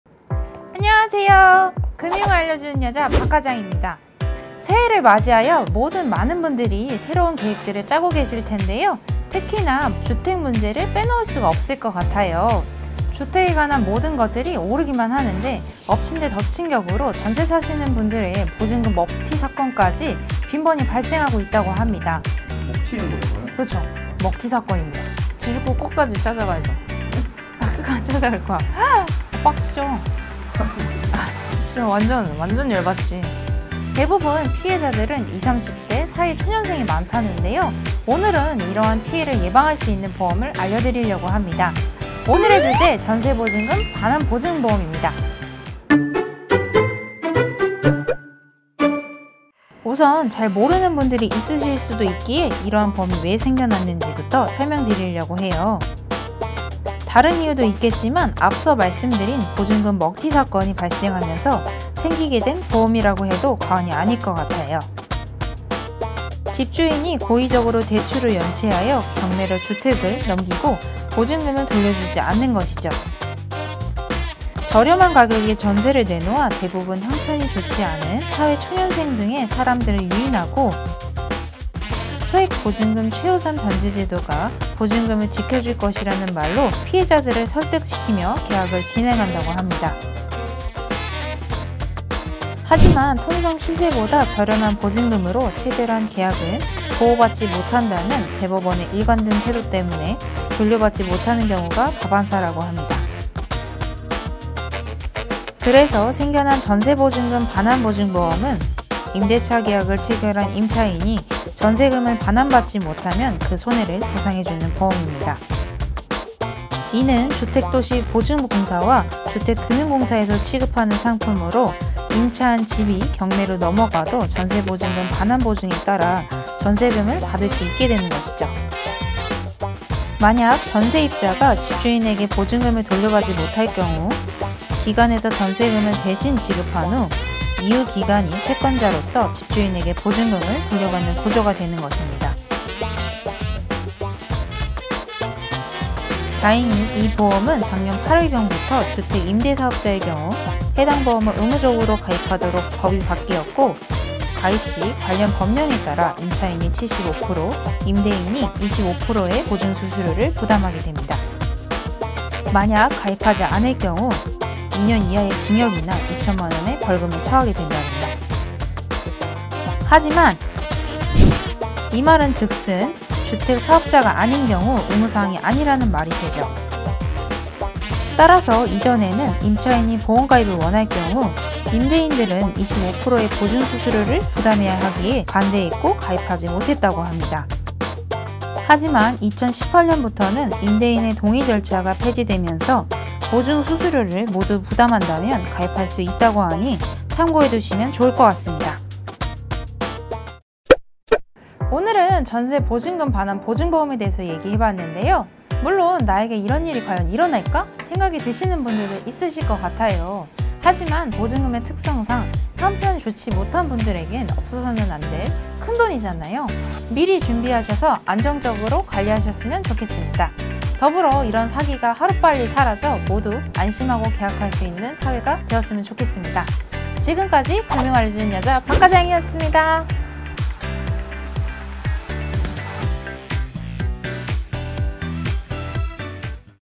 전세사기 예방법, 전세보증보험으로 내 전세금 지켜보세요!
한 때 '신종전세사기'가 큰 이슈였죠? 저렴한 가격에 전세를 내놓고, 형편이 좋지않은 사람들에게 보증금을 지켜줄거란 말로 설득시키며 계약을 진행한다고 합니다.
오늘은 이러한 피해를 예방할 수 있는 보험에 대해 알아보려고하니
영상 끝까지 시청해주시고 댓글과 좋아요는 저희에게 큰 힘이 됩니다^^

00:00 - 인트로
00:43 - 오늘의 주제
00:54 - 보험 출시 이유
01:49 - 전세보증금 반환보증보험